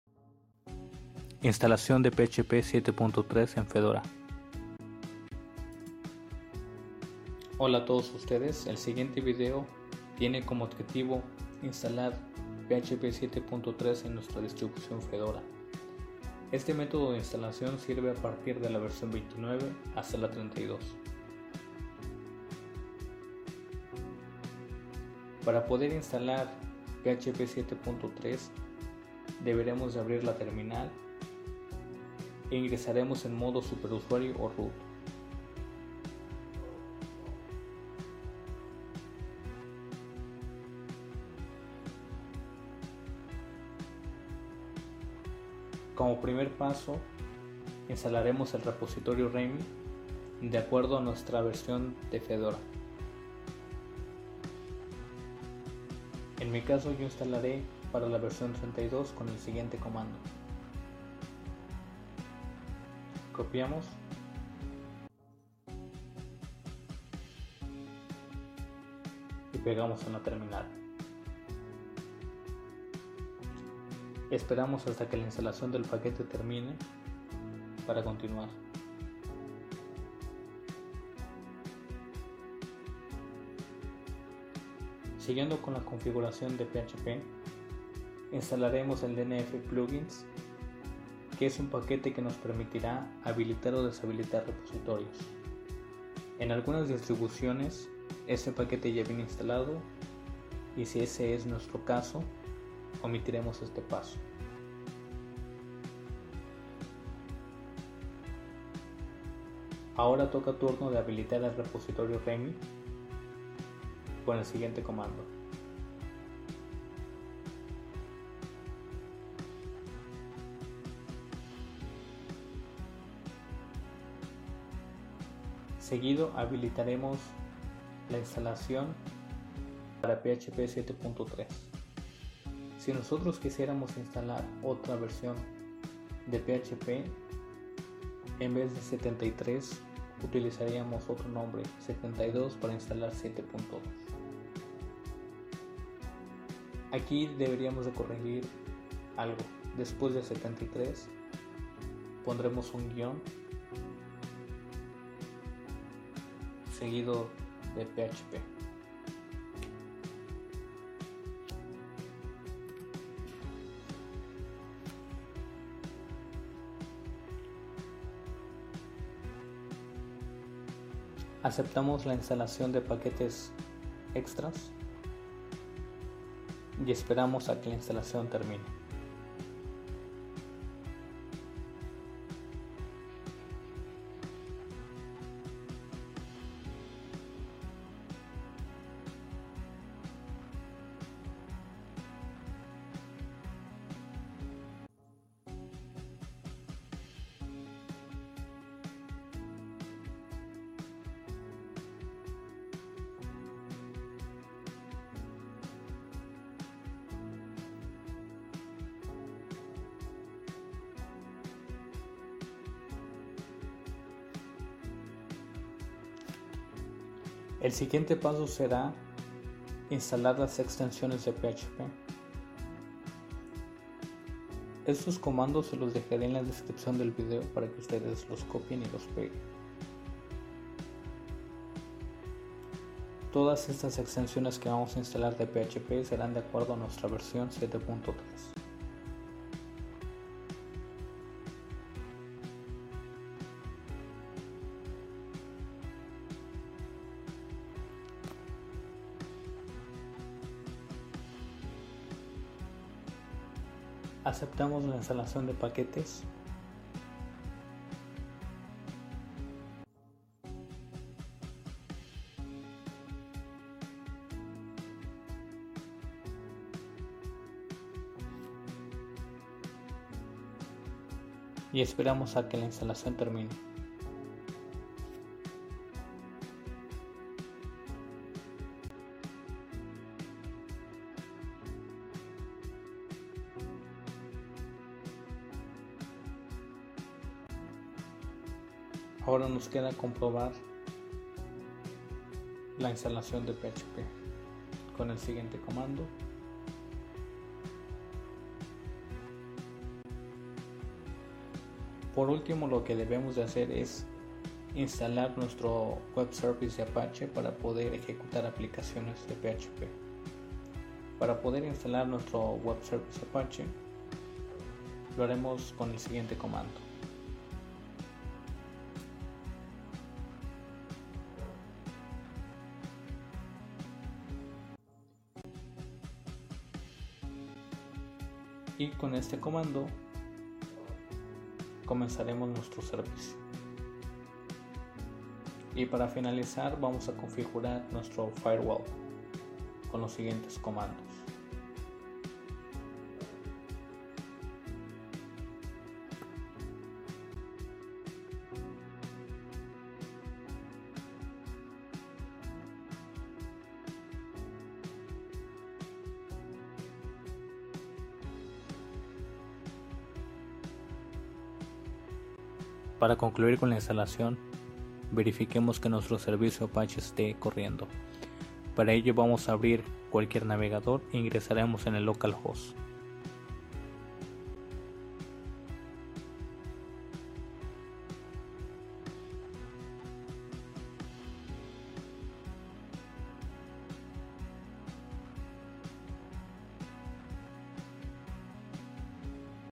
Fedora | Cómo instalar PHP 7.3 y Apache en Fedora Linux
Como INSTALAR PHP 7.3 y el Servidor Apache en Fedora

El objetivo del video es mostrar la instalación del servidor Apache & PHP 7.3

Requisitos
[ Instalación de "REMI Repository" ]
# Add Remi repository to Fedora 32:

[ Instalación de DNF Pluggin si no esta instalado ]
➡ dnf -y install dnf-plugins-core

[ Habilitar REMI Repo ]
➡ dnf config-manager --set-enabled remi

[ Instalar PHP 7.3 ]
➡ dnf --enablerepo=remi install php73 httpd php73-php-common

[ Instalación de las extensiones de PHP 7.3 ]

[ Comprobar instalación de PHP ]
➡ php73 -v

[ Instalación de Apache Service  ]
➡ dnf install httpd
➡ systemctl start httpd.service

[ Configurar del Firewall ]
➡ firewall-cmd --get-active-zones
➡ firewall-cmd --permanent --zone=public --add-service=http
➡ systemctl restart firewalld.service